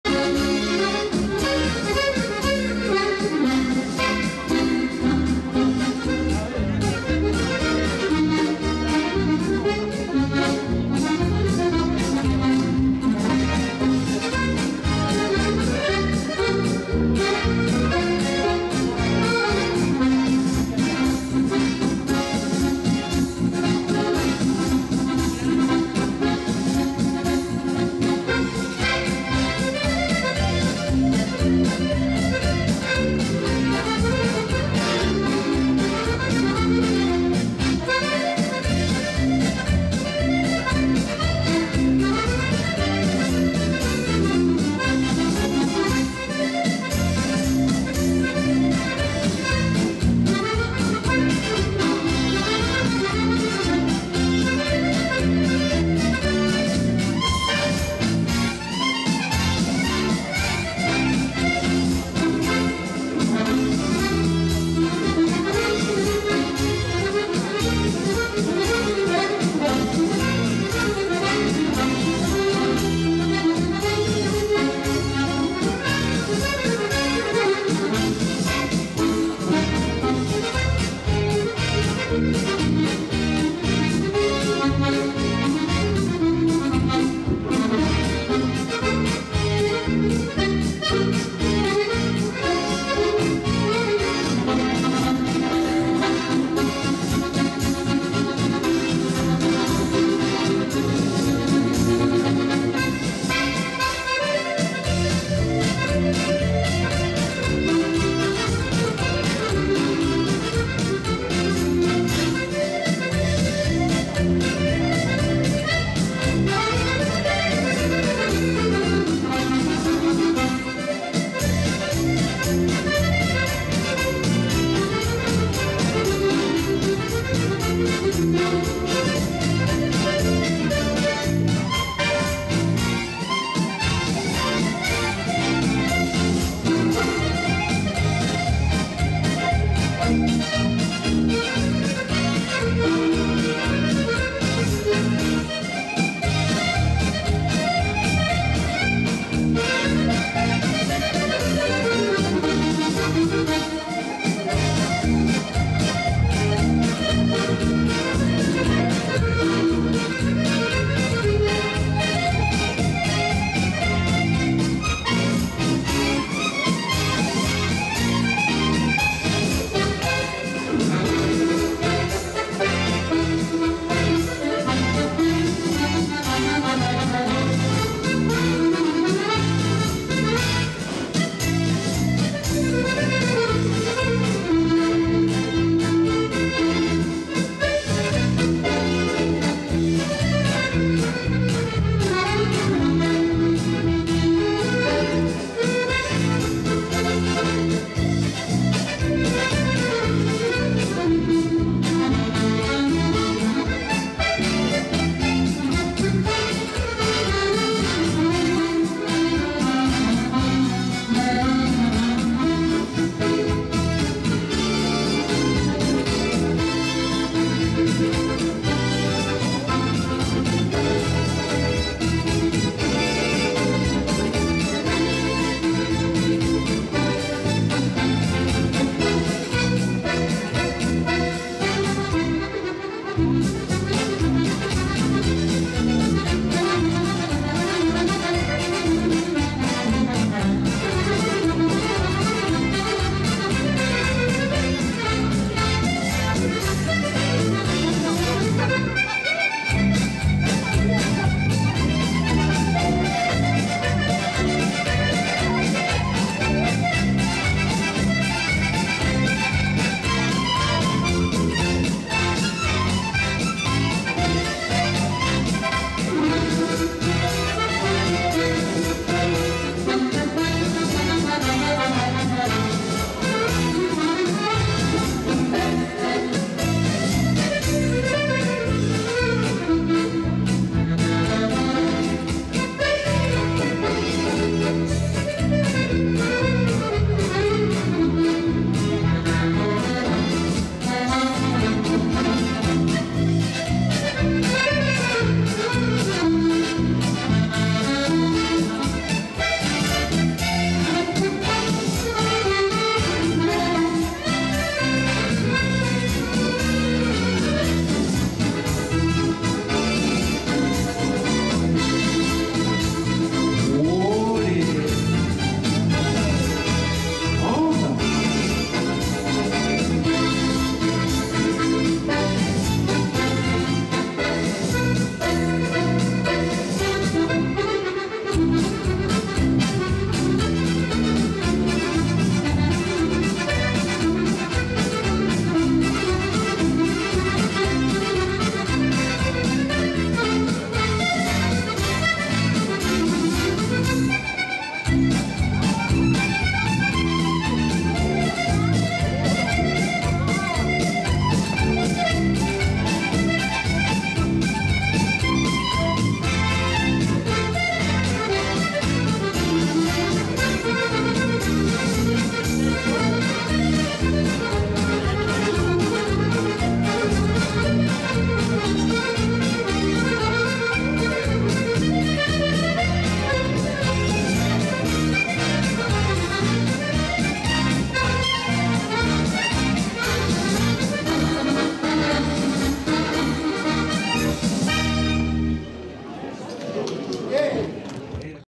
Jean François BLANC au bon temps Aurillac sept 2024 Pasodobles
Jean-François Blanc au bon temps Aurillac sept 2024 - vidéo et réalisation Alexandra Astorg (Sandy)